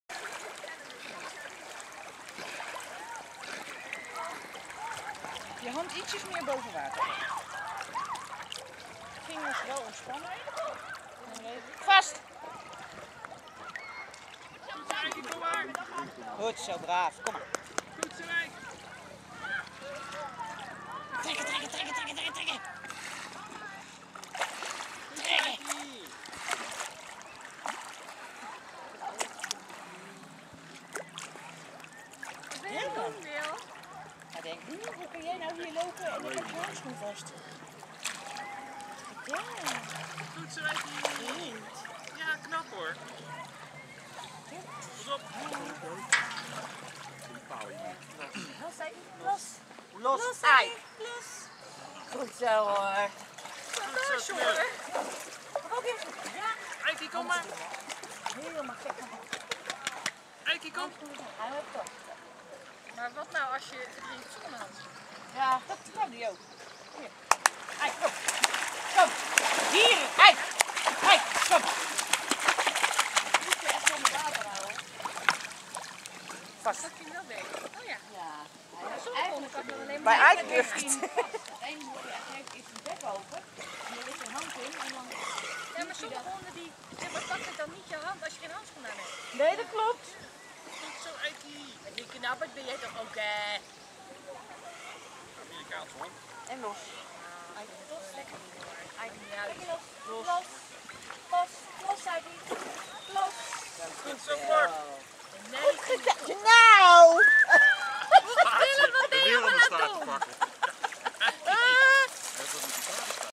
NF Kringgroep 's-Gravenhage watertraining 2013. Ike
Drenkeling redden door bij de hand te pakken.